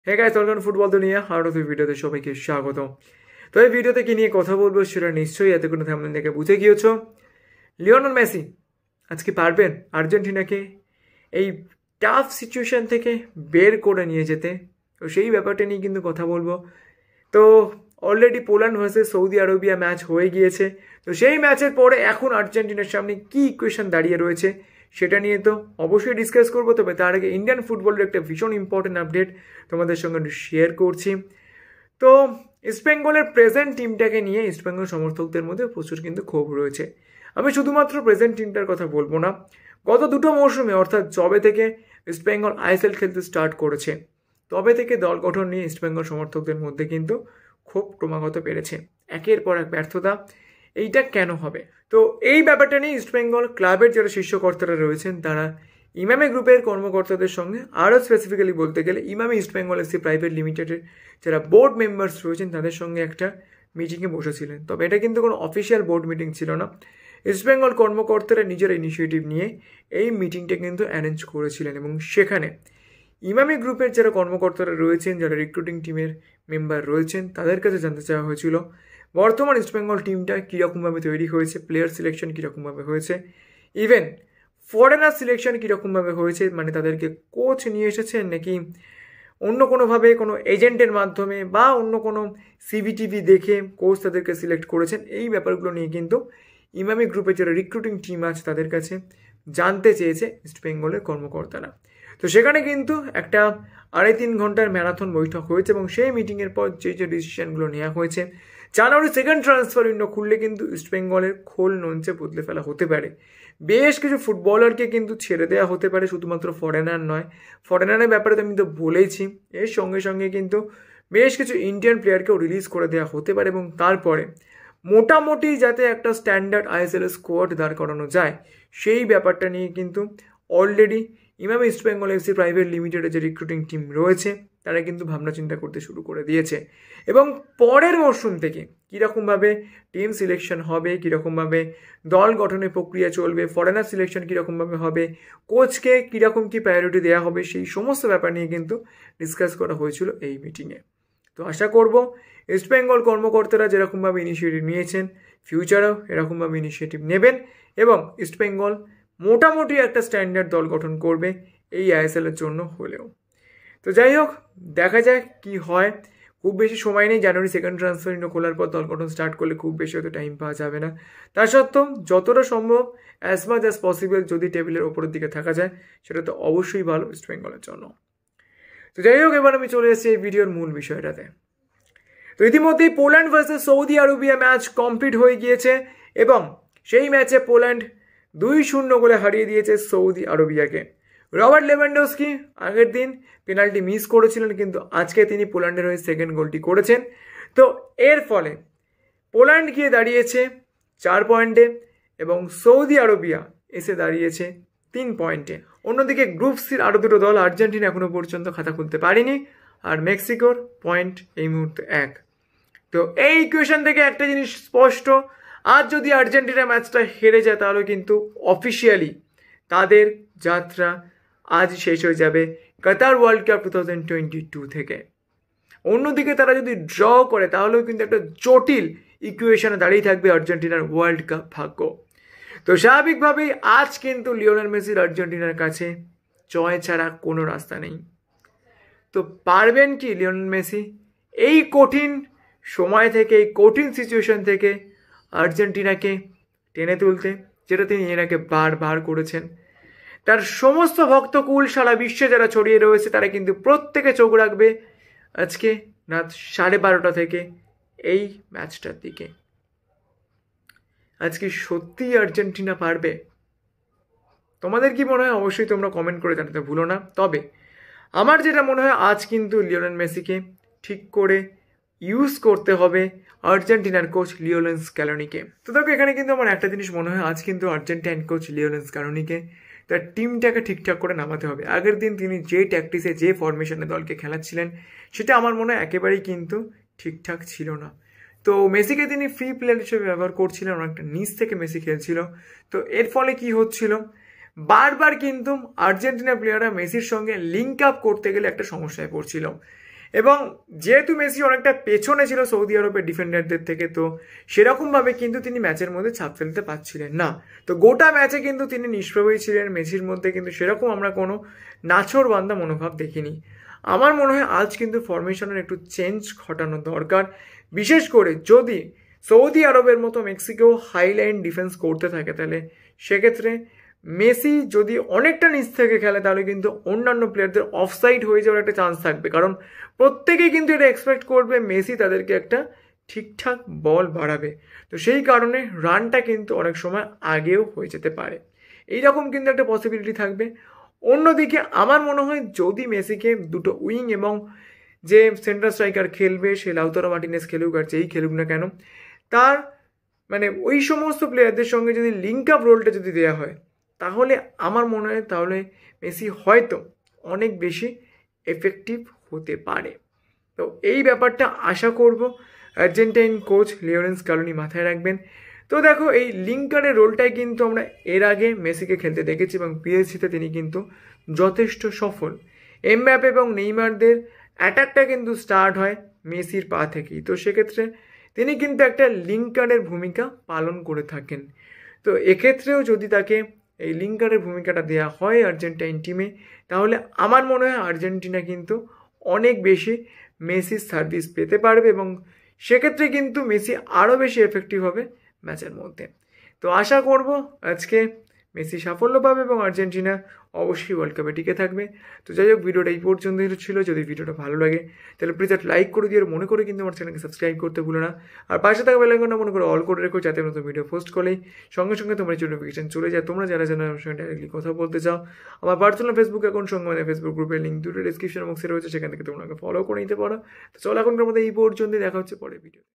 জানুয়ারিতে দলের খোলনলচে বদলে ফেলার সিদ্ধান্ত নিল Emami-EastBengal🔥আজ পারবেন কি Lionel Messi?🤔
PLEASE FOLLOW MEbu